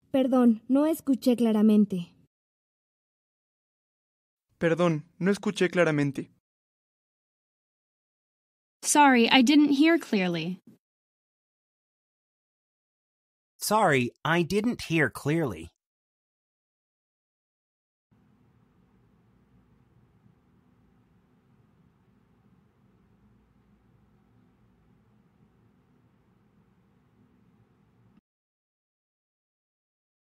How to say "Sorry, I didn't hear clearly." in Spanish
Male Voice:
Perdón, no escuché claramente.

Male Voice Translation:
Sorry, I didn't hear clearly.